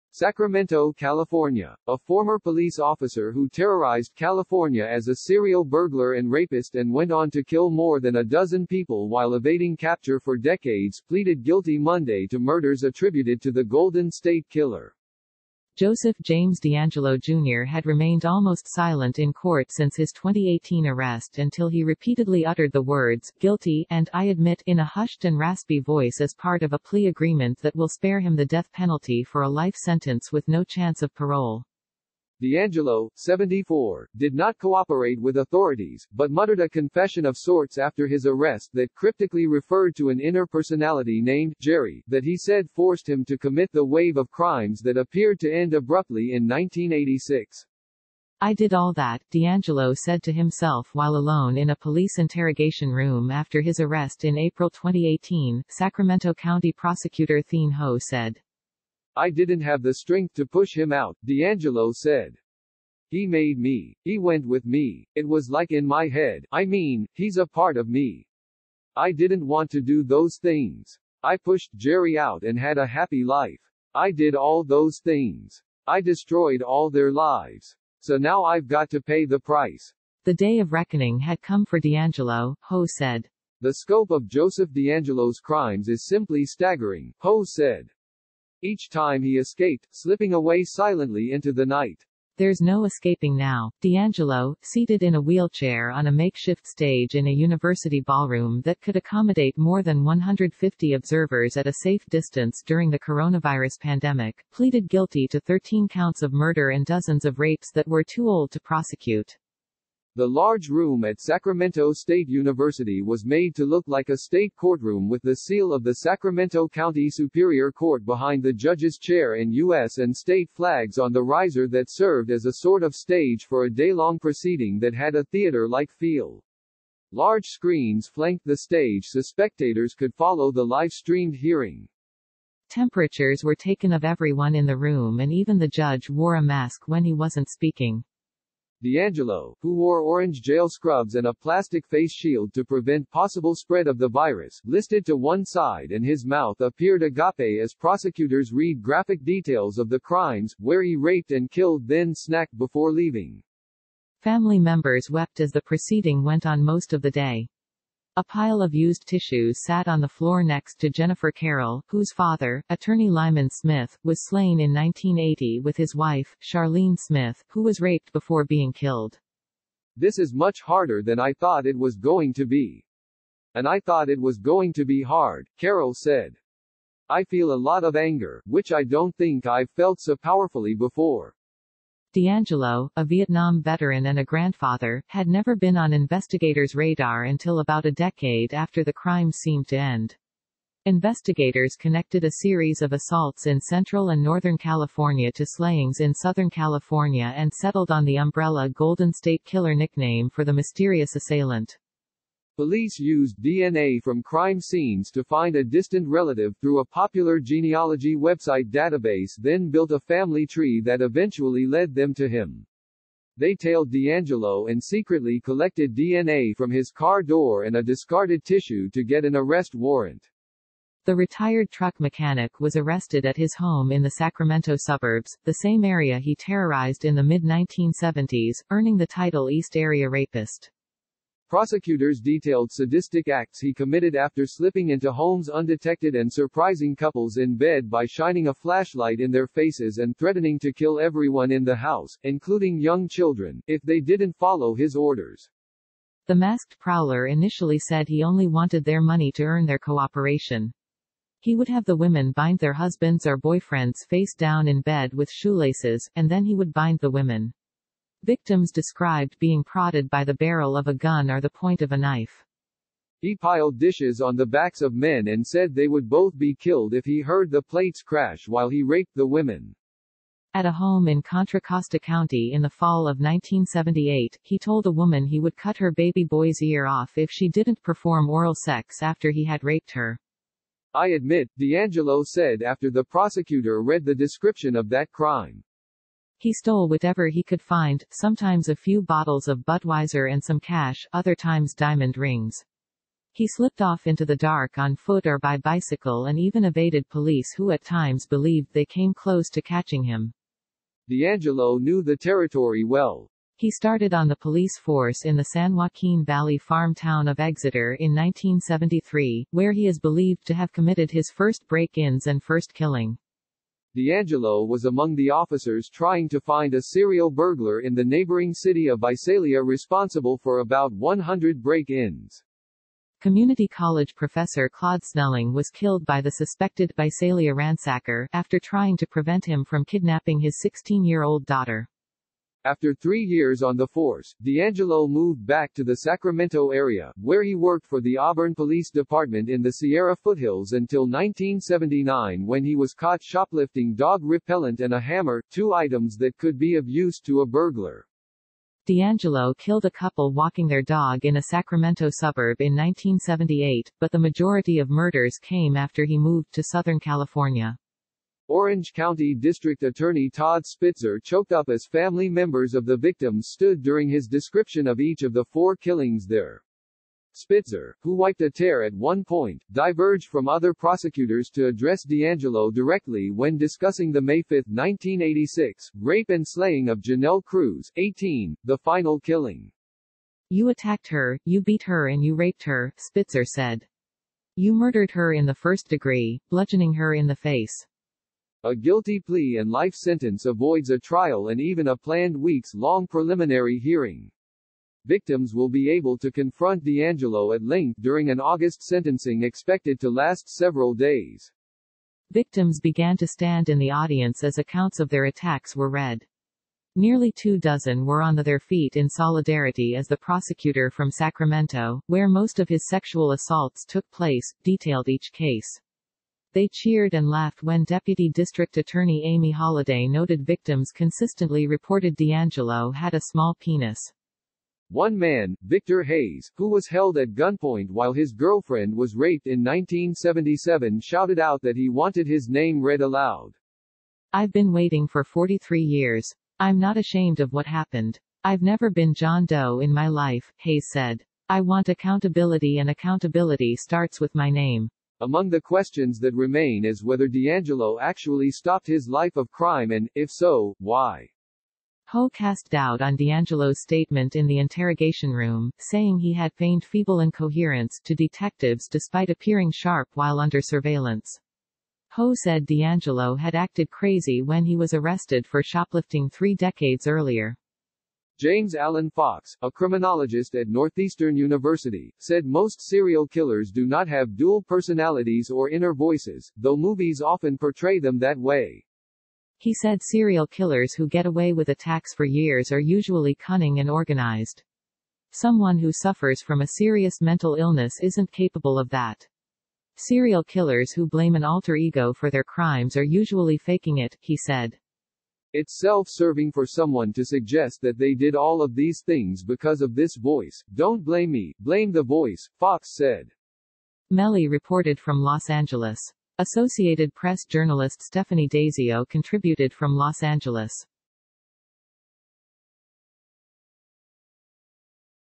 Golden State Killer pleads guilty, admits to dozens of rapes, murders
SACRAMENTO, CALIF. — A former police officer who terrorized California as a serial burglar and rapist and went on to kill more than a dozen people while evading capture for decades pleaded guilty Monday to murders attributed to the Golden State Killer.